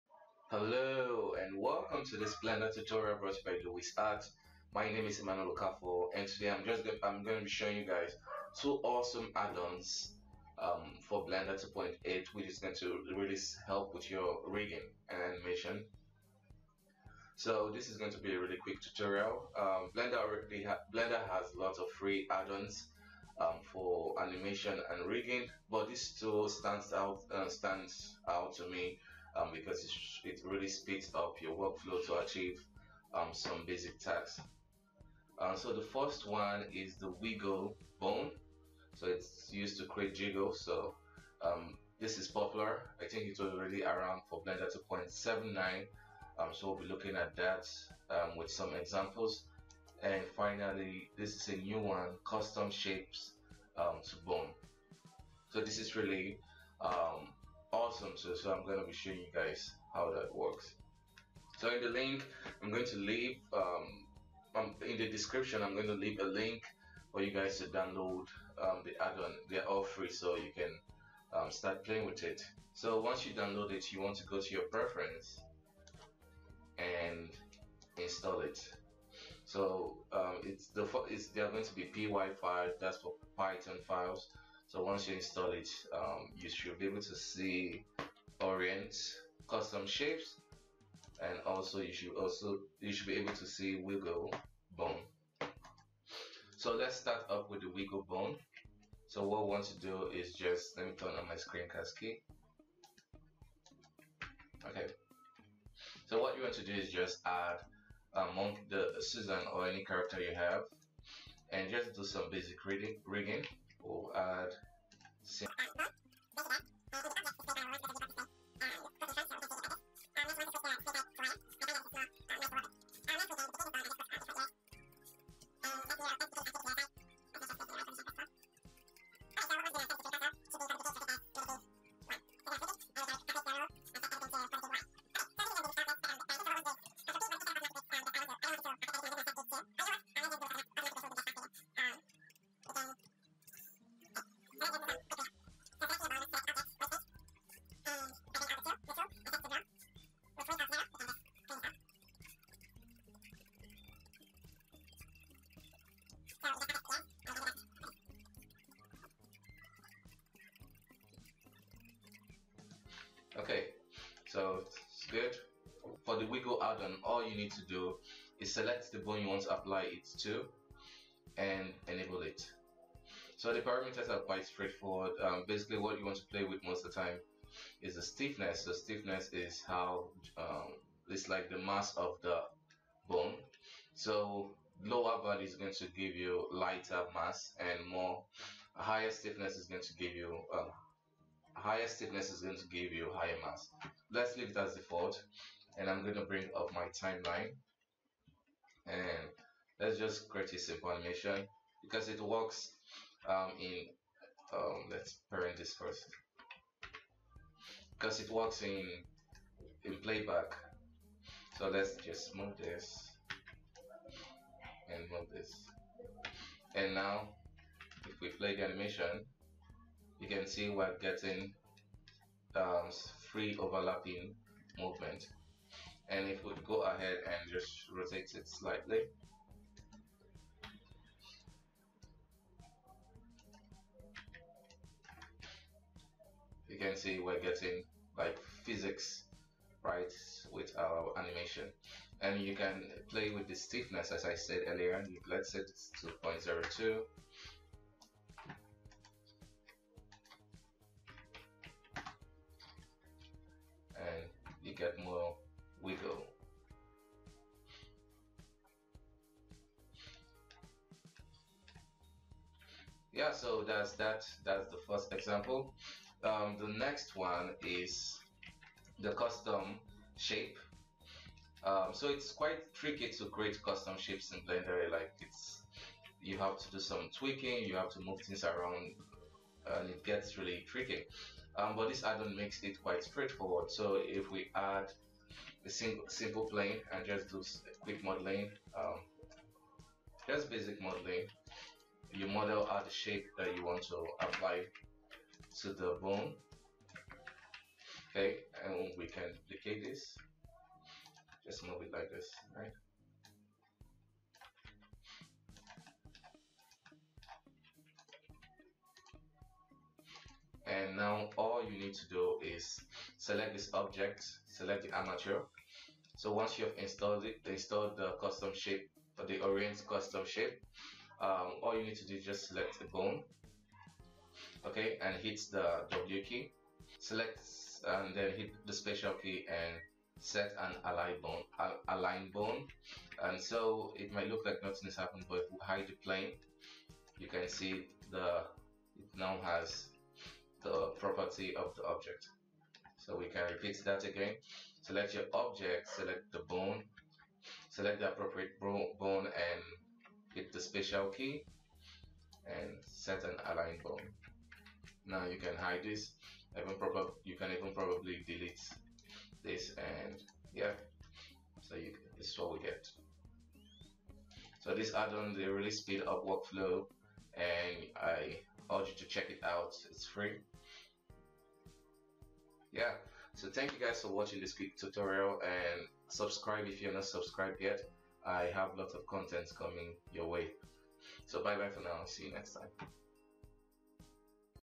2 Helpful Addon for Rigging and Animation in Blender 2.8X
in this blender tutorial, i will be showing you guys how to use this free addon in blender. i will be sharing with you Technics to get the best out of it

im also available for freelance work !!